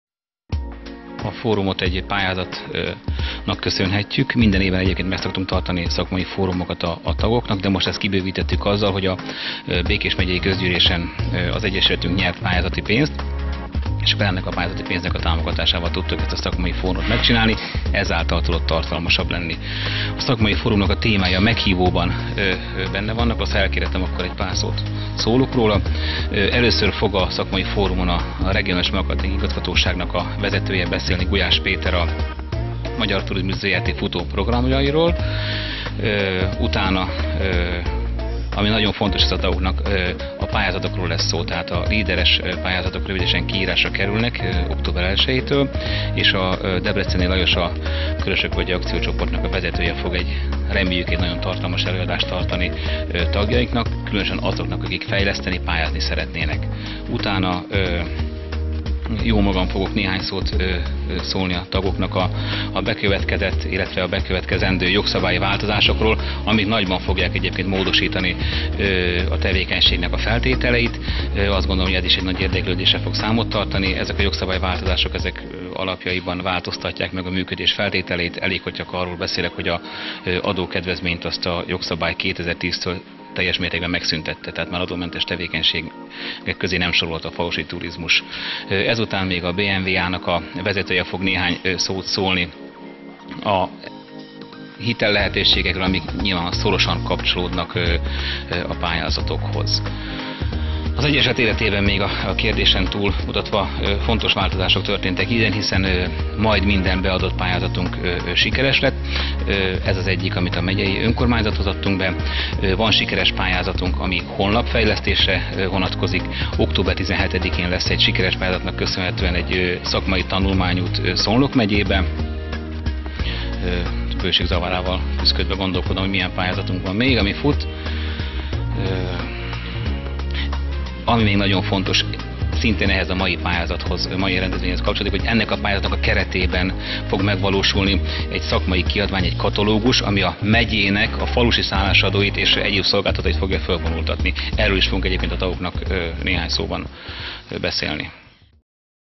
Szakmai fórum a Békés megyei Falusi Turizmus Egyesület szervezésében Szarvason
A Békés Megyei Falusi Turizmusért Egyesület szakmai fórumot szervezett Szarvason. Frankó Pál, az egyesület elnöke a Körös TV stábjának nyilatkozva elmondta, hogy a fórumot a Békés Megyei Közgyűlés által kiírt pályázatnak köszönhetik.  A szakmai összejövetel témái: a Magyar Turizmus Zrt. programjai; a Leader-es pályázatok; jogszabályi változások; hitellehetőségek. Frankó Pál a továbbiakban kiemelte a fórum fontosságát, mint kapcsolattartó és előremozdító színteret, lehetőséget.